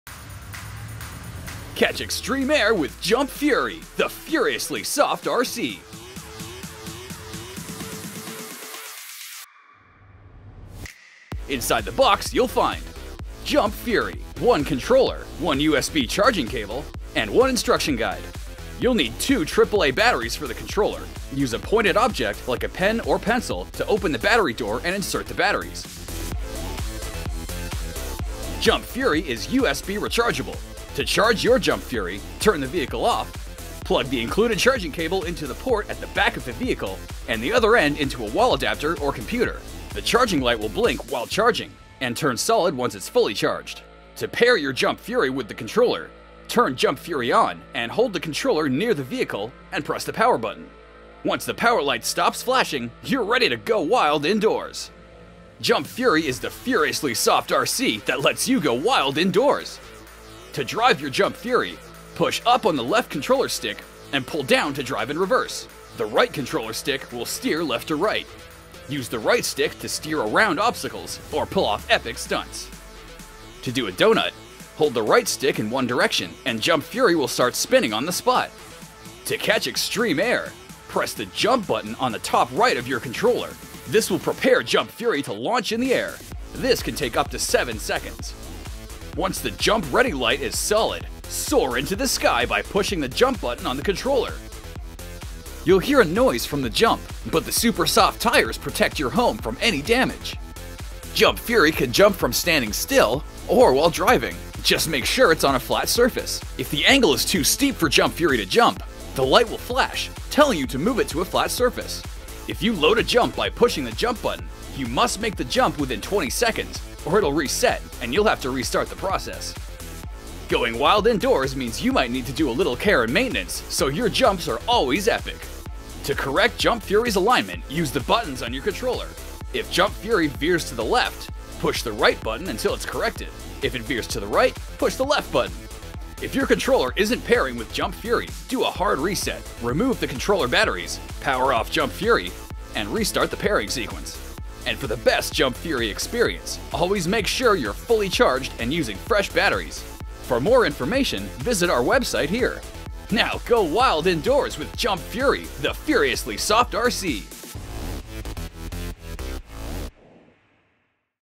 Air Hogs Learn how to Jump with Jump Fury! Seriously Soft RC!
Learn how to Jump with Jump Fury! It’s the furiously fast and seriously soft RC from Air Hogs! Learn how to catch extreme air and pull off epic stunts!

Catch extreme air with Jump Fury, the all-new innovative RC truck from Air Hogs Super Soft! Takes remote control trucks to new heights and jump up to 22 inches high with this all-new innovative RC truck. Air Hogs Jump Fury puts insane speed and precision stunt driving in your hands. Jump on, over and on top of the competition! Go wild indoors and master epic stunts including 360-degree spins, flips, drops, rebounds and more!